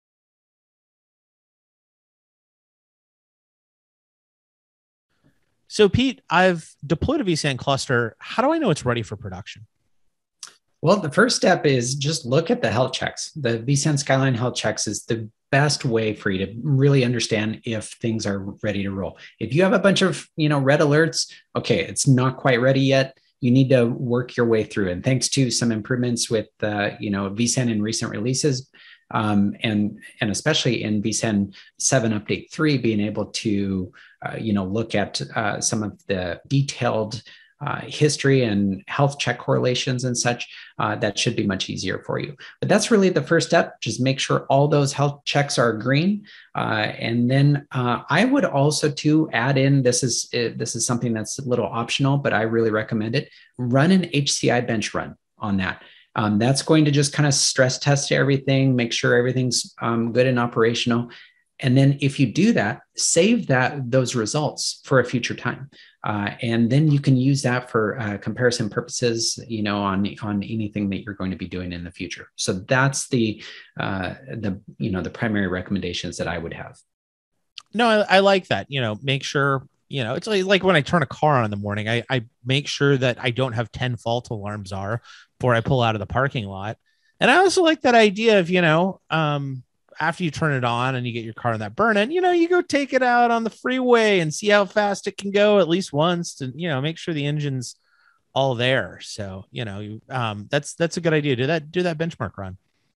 vSAN Quick Questions - How do I verify that a new vSAN cluster is ready for production?
How do I verify that a newly provisioned vSAN cluster is ready for production?